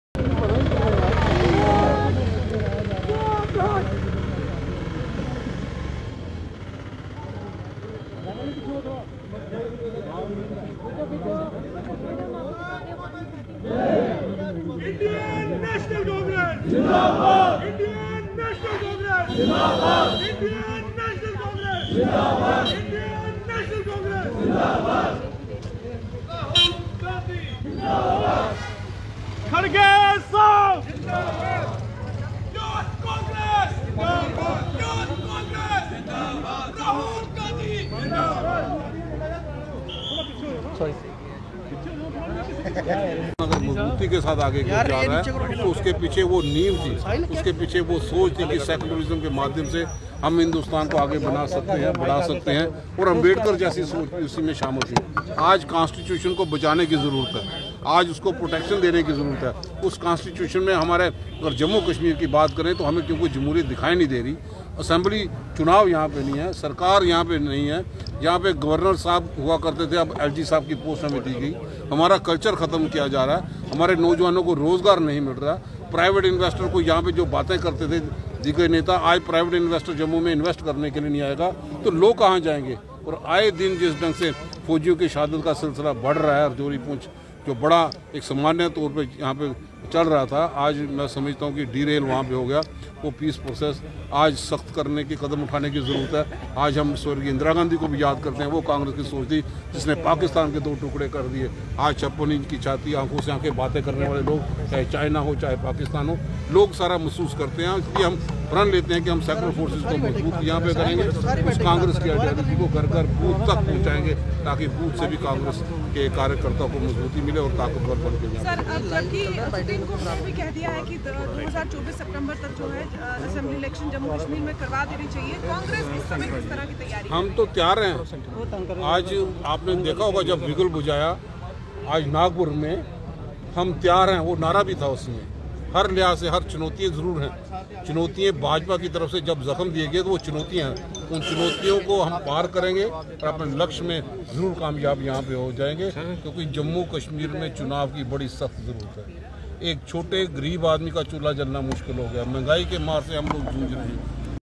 Indian National Congress celebrated its Foundation day at Jammu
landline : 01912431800